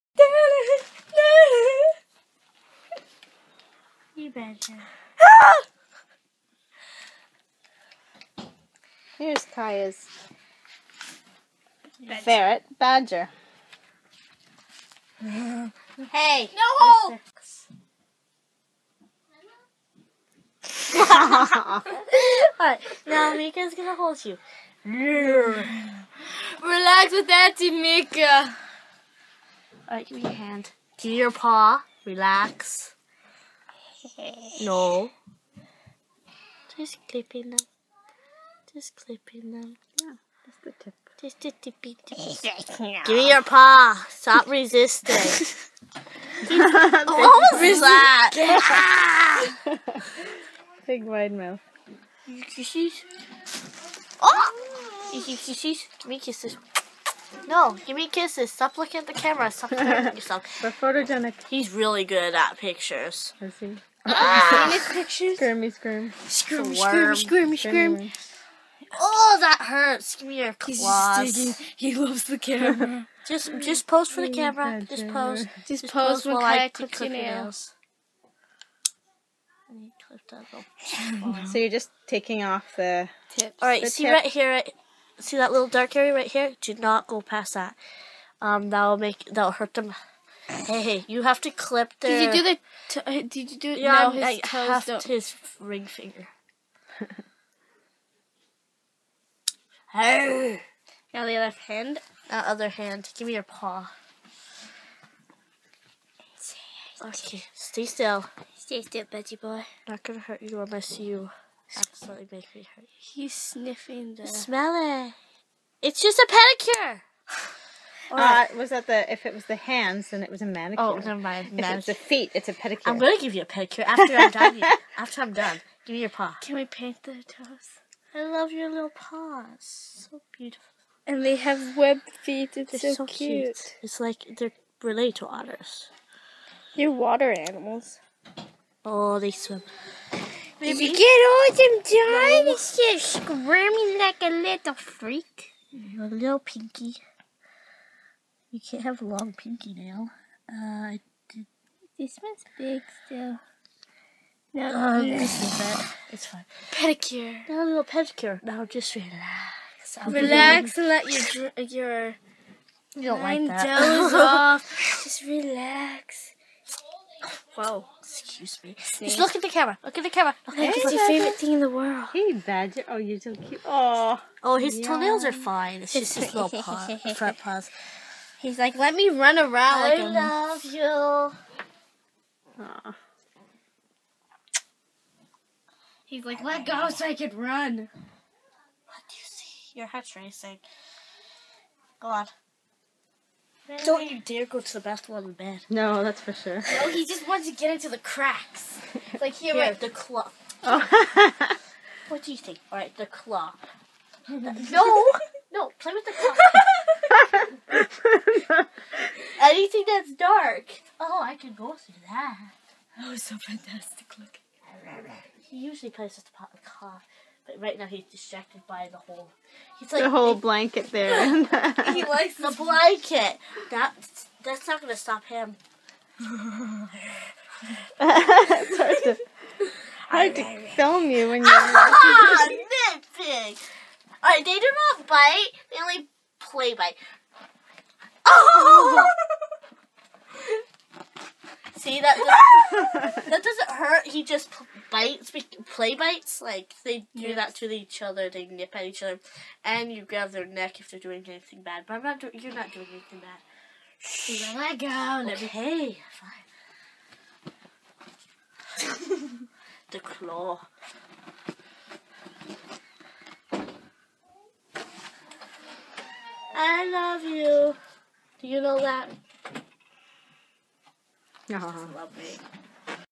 Clipping Badger The Ferret's Claws-Vlogmas Day 11
Kaiya clips Badger the ferret's claws while Mika helps hold him. He also gets in some playtime!